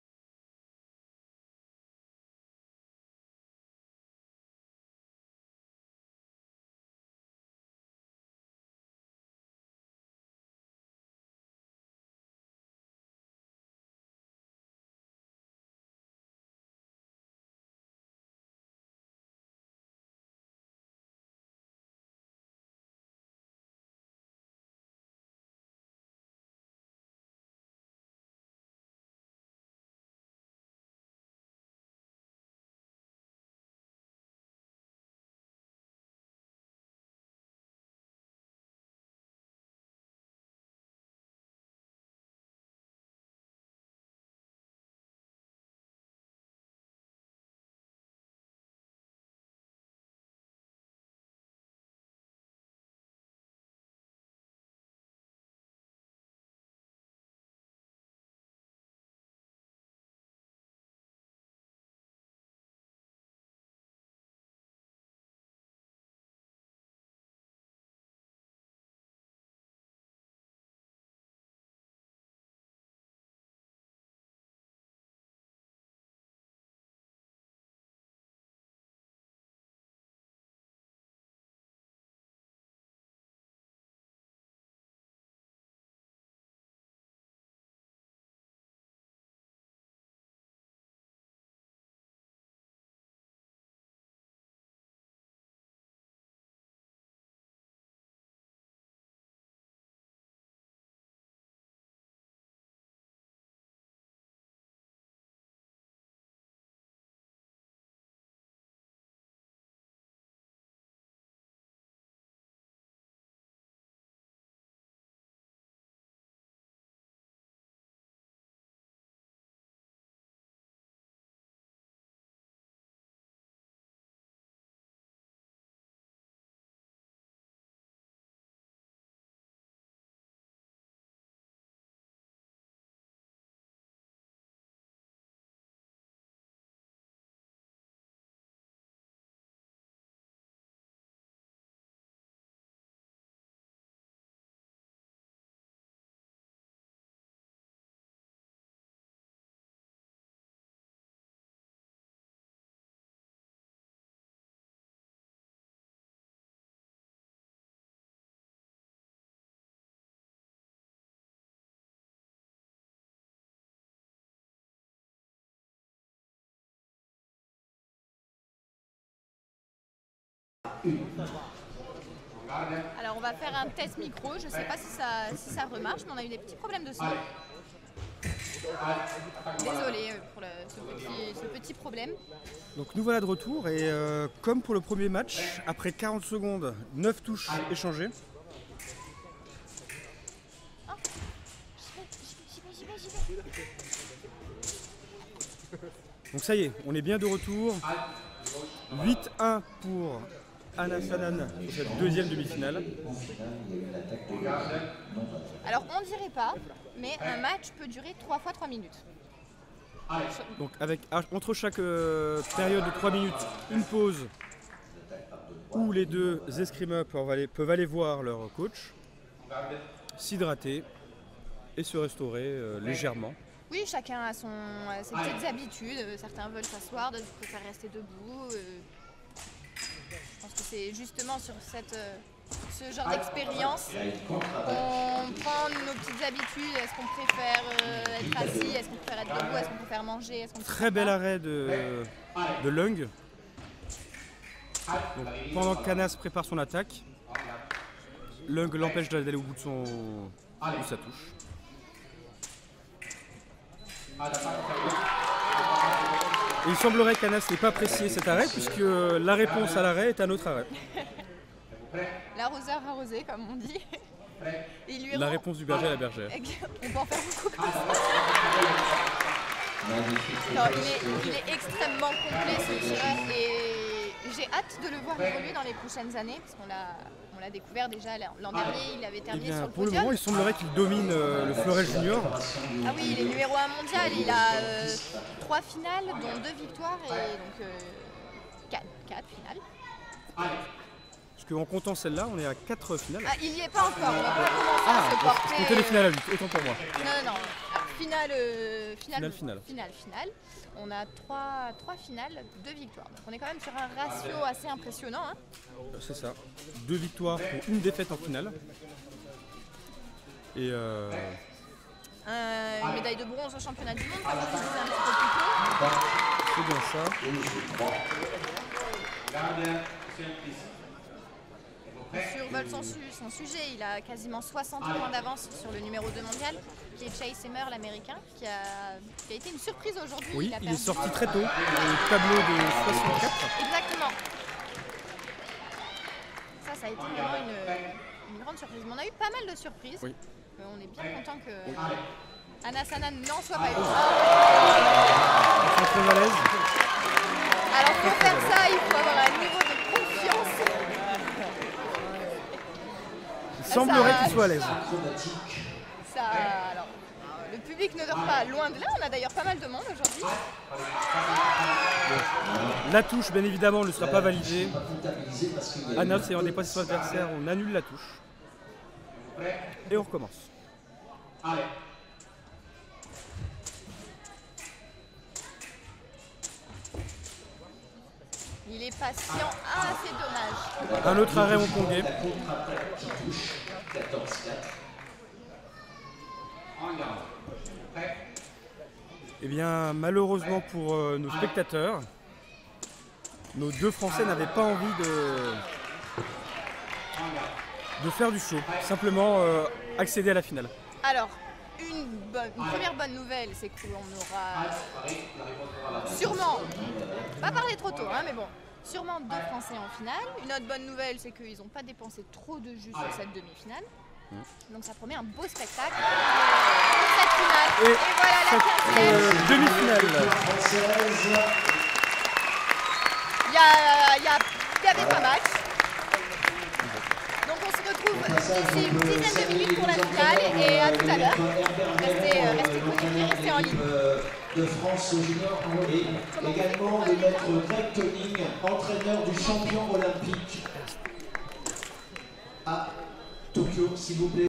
Aix en Provence World Cup 2023 JMF - L4 - Anas Anane FRA v Leung Chin Yu HKG
This is the second semifinal bout at the 2023 junior men's Foil World Cup in Aix en Provence, France. Anas Anane of France is on the left and Leung Chin Yu of Hong Kong is on the right.